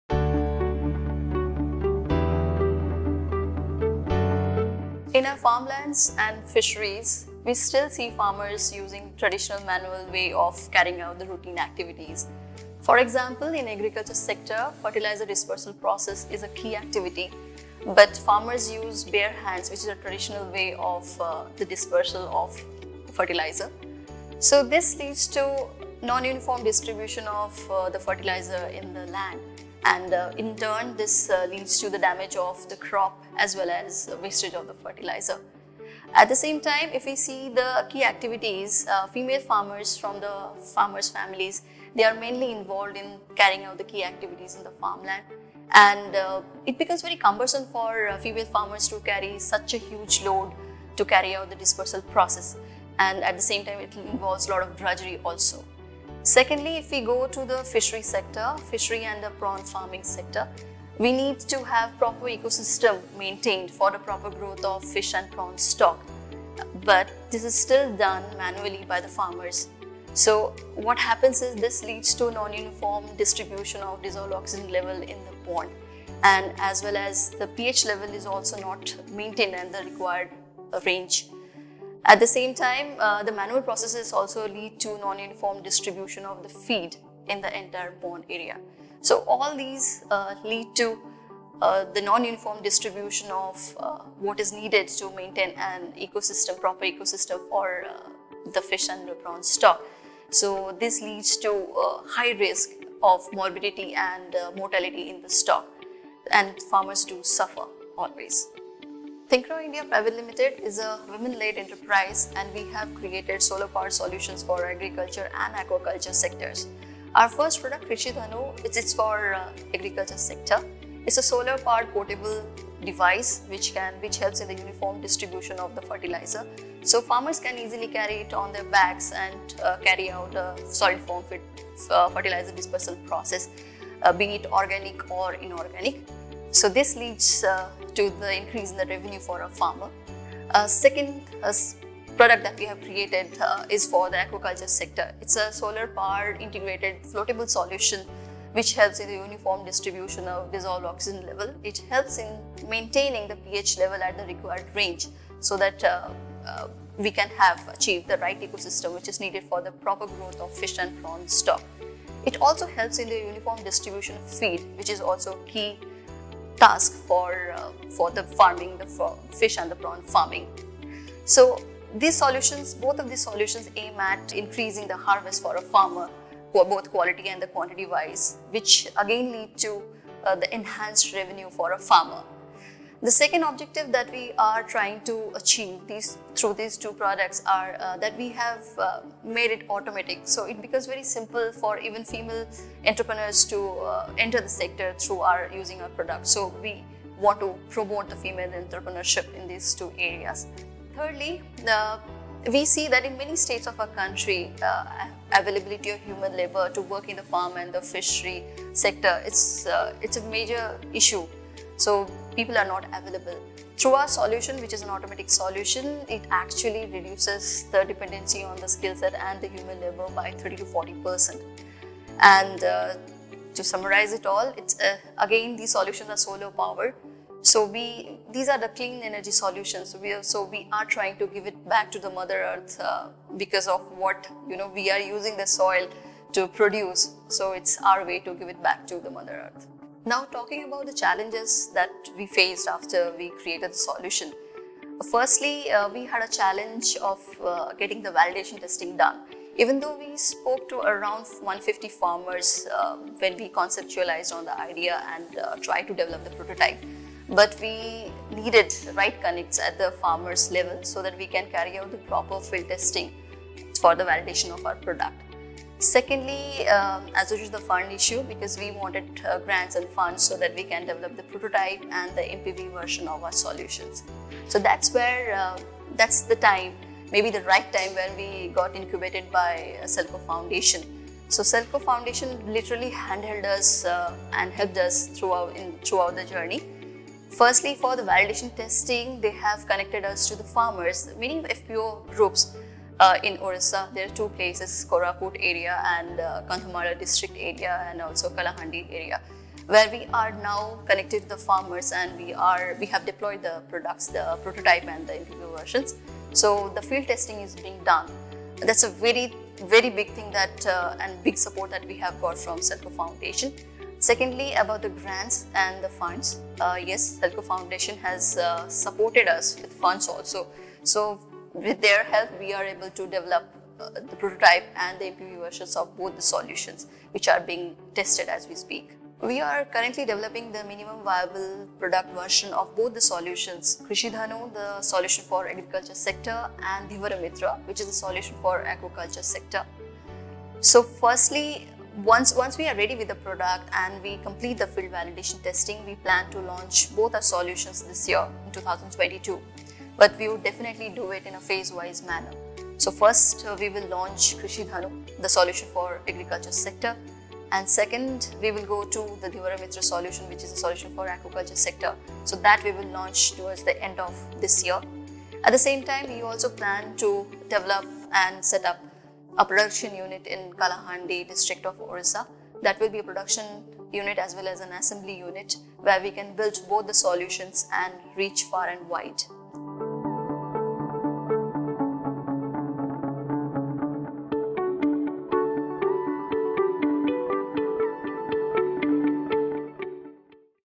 Solar powered IoT enabled innovations for farmers
The innovative solar-powered products by Thinkraw India are the technological interventions that can reduce the hardship of farmers and increase productivity. 'Krishidhanu' is an IoT-enabled solid fertilizer dispenser that can be used by women instead of dispensing by hand. 'Dhivaramitra' is an integrated solar power-operated floating device for fish and prawn farming that helps in the uniform distribution of feed and maintains uniformity of dissolved oxygen level and desired pH level in a body of water. This is an alternative to diesel-based aerators used by fish farmers because of an uninterrupted power supply which adds to the cost.